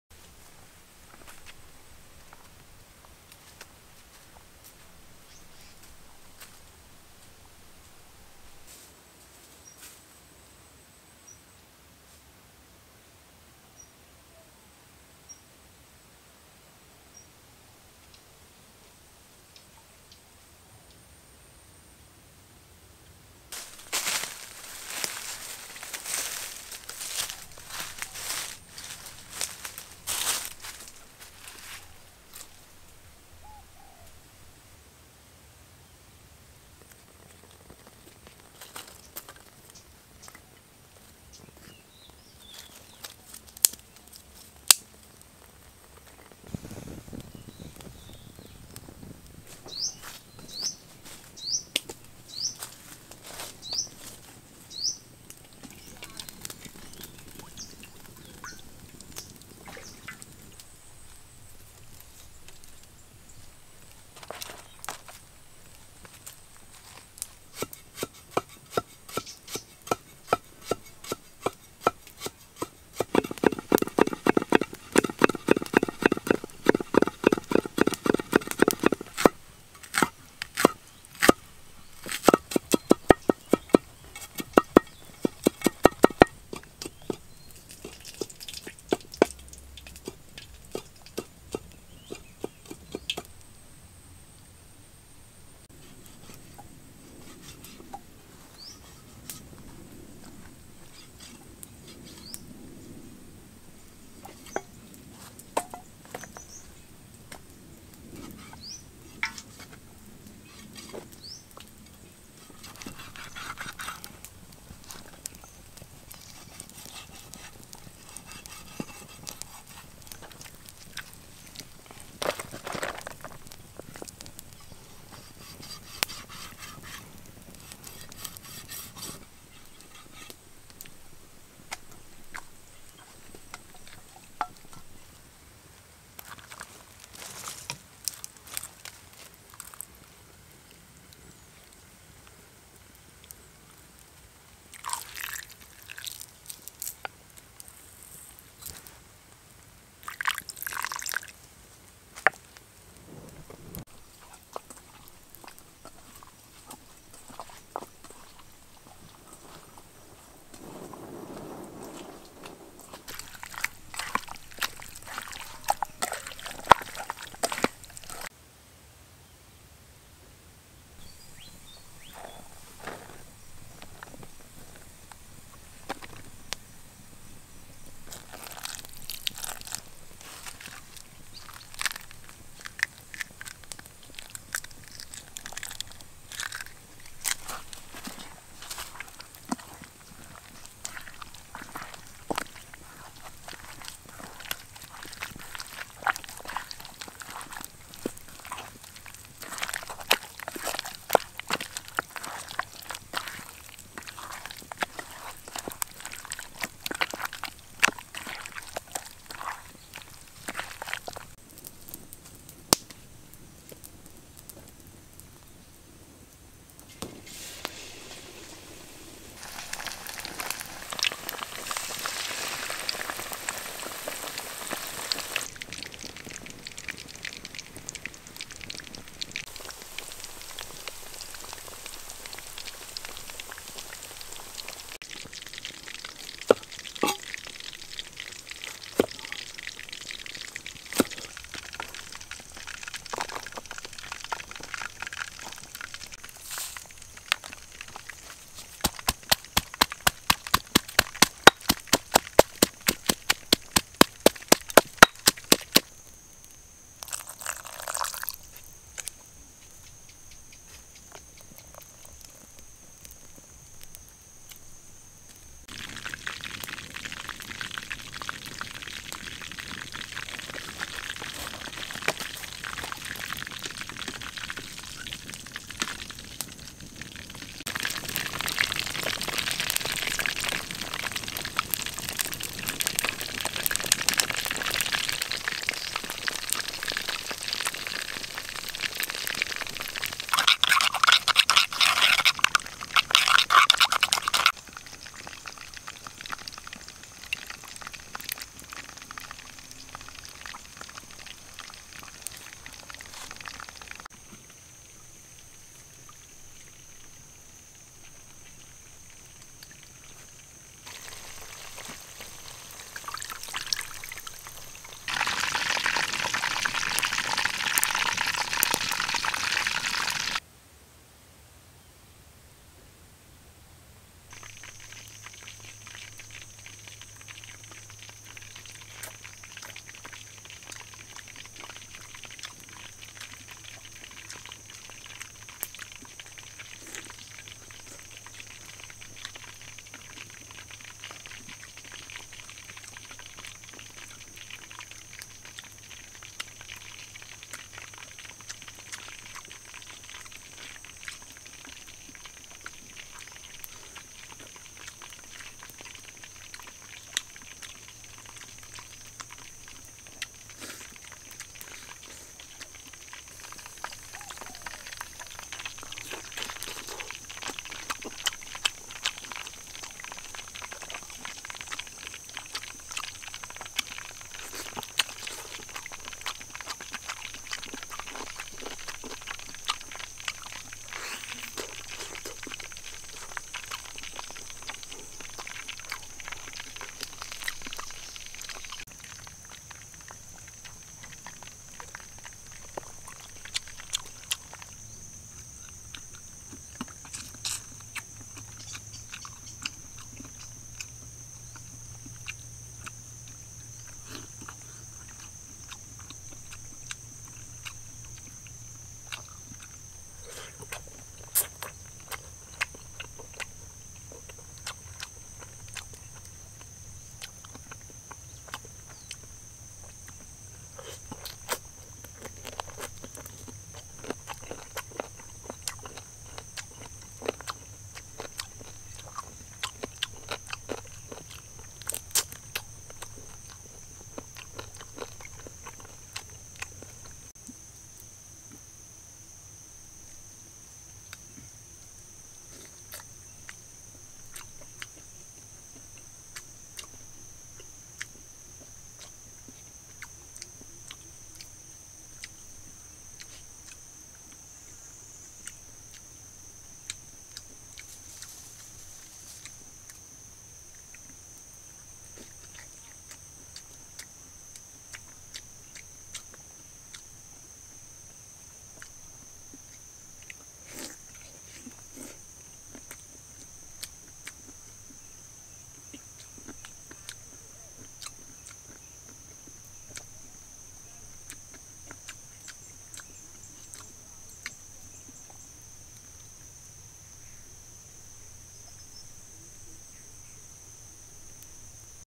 Cooking Crispy Chicken Drumstick with My Secret Ingredient At Countryside Kitchen
Cooking Crispy chicken leg with My Secret Ingredient At Countryside Kitchen. We usually visited the rice field next to our village. So at that point we prepared and began cooking the side of pork there. We usually used red chili, purple white garlic, lemongrass paste, lemon leaf, oyster oil, scallion, salt, cane sugar, eggs, tamarind, soy and sweet fish sauce. we also added more spicy seasoning and native homemade sauce. We had the food ready and joyful eating with a very nice day along the wide green rice field so tasty.